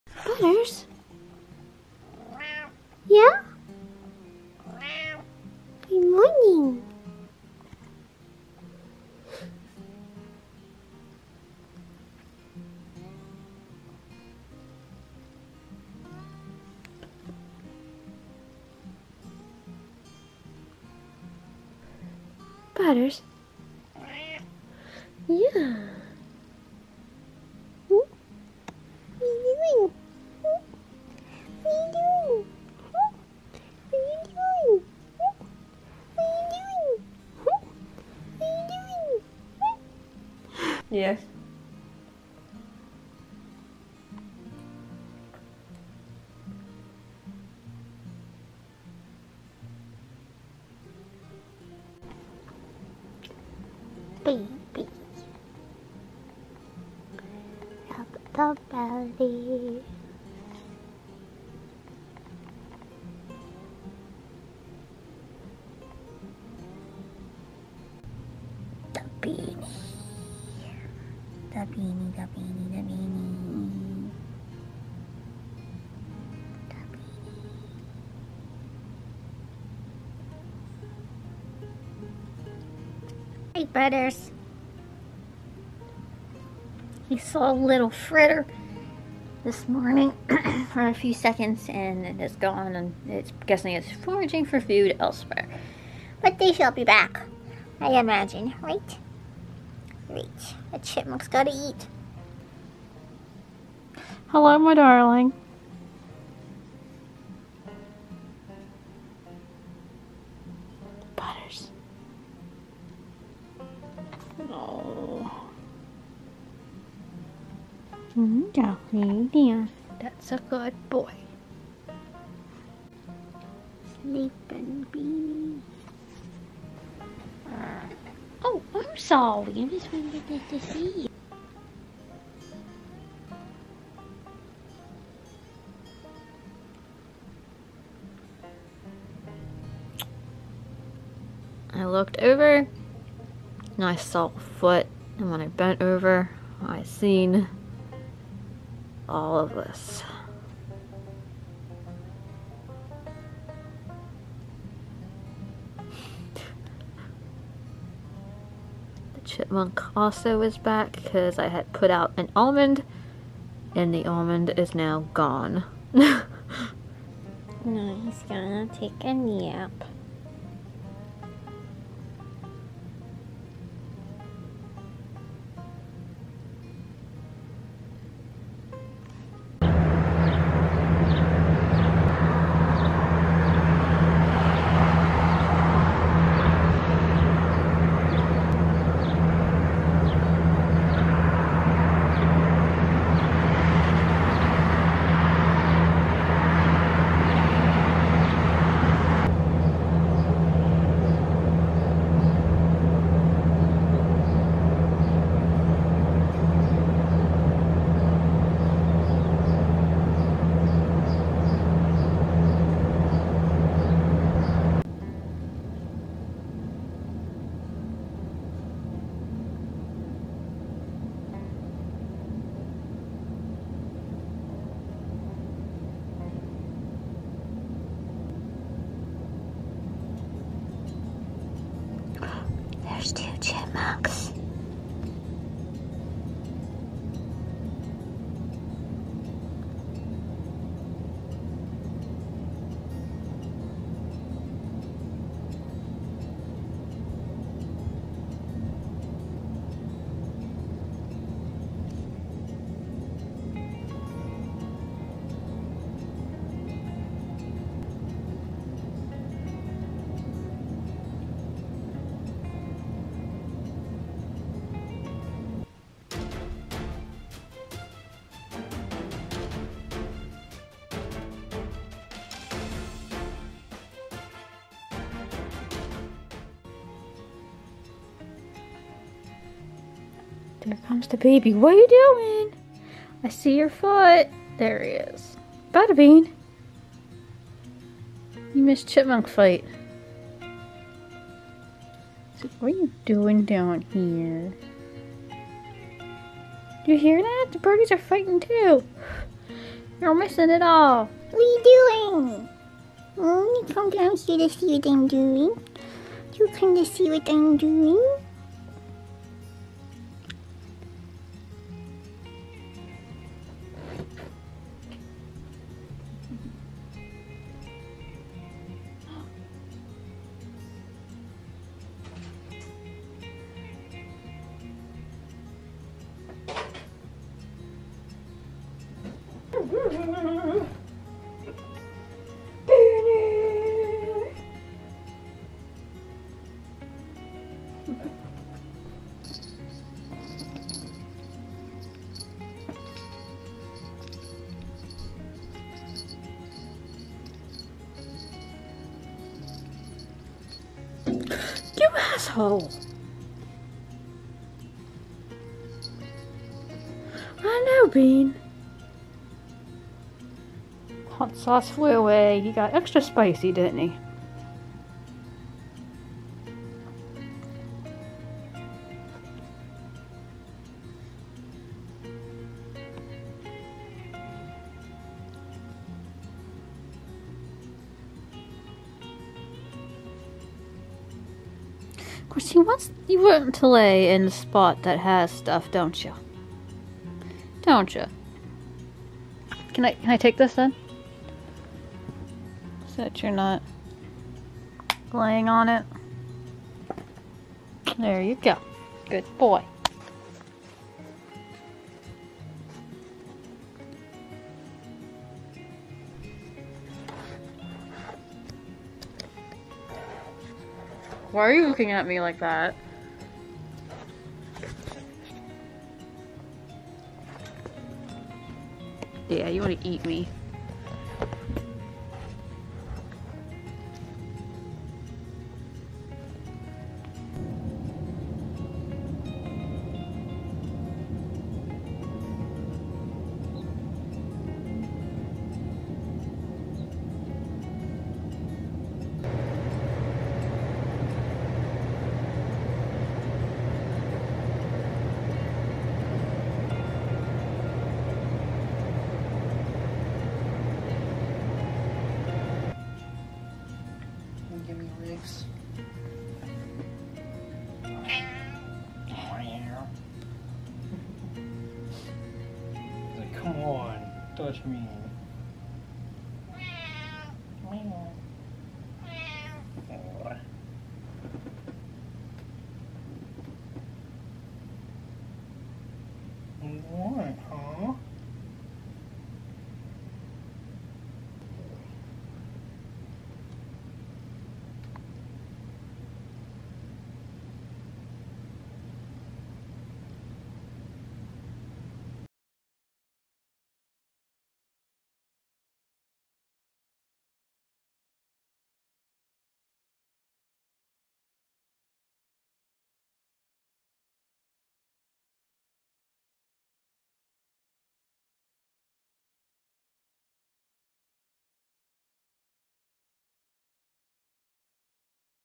A day of lounging and watching wildlife
The chippies fight each other for food...drama!